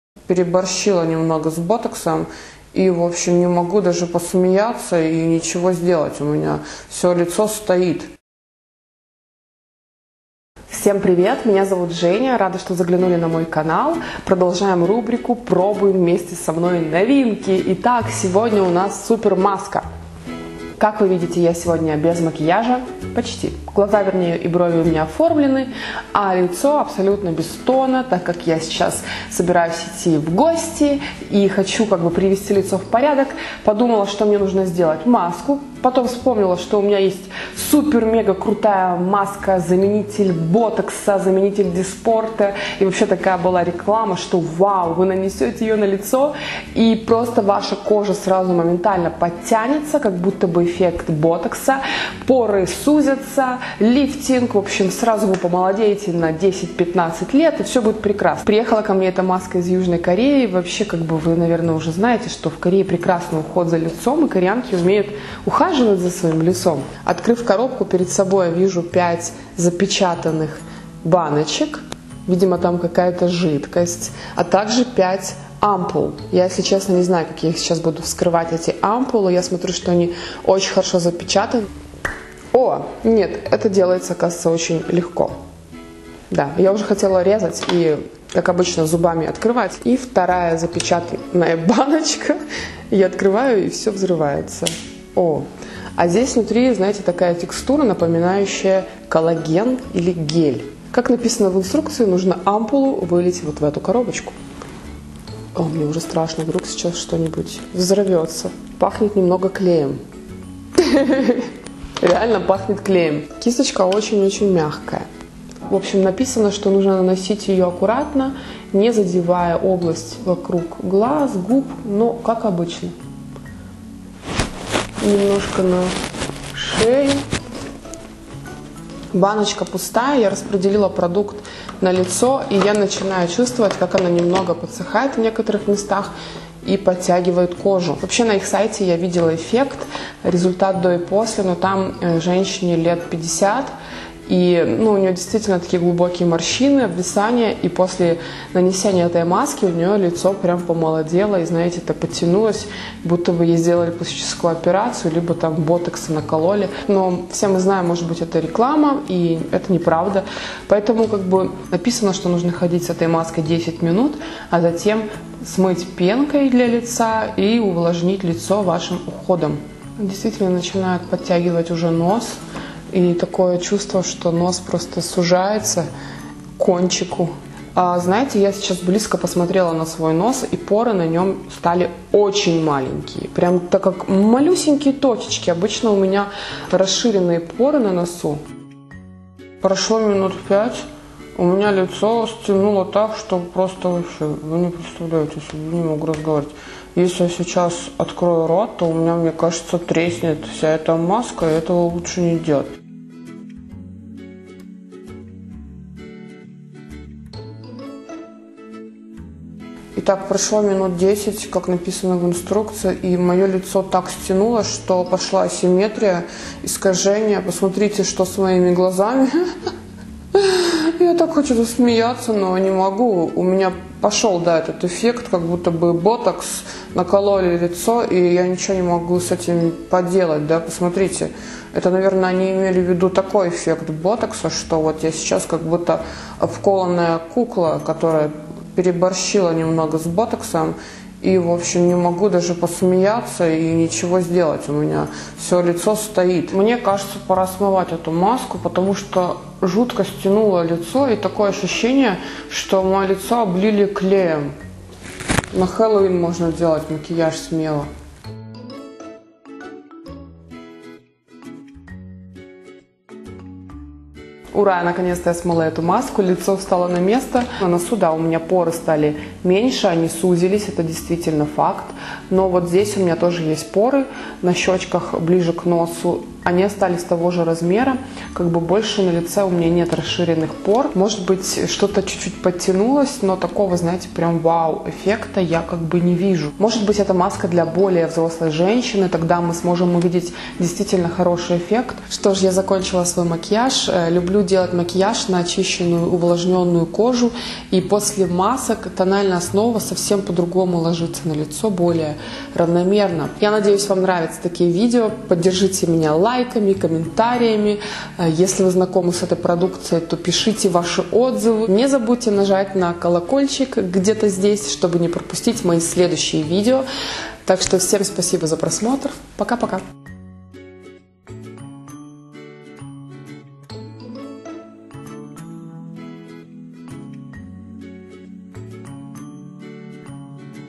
МАСКА С ЭФФЕКТОМ БОТОКСА... ЛИЦО ПОЕХАЛО)))
Привет-Привет) Меня зовут Женя Гейн сейчас я буду тестить вместе с вами новую маску!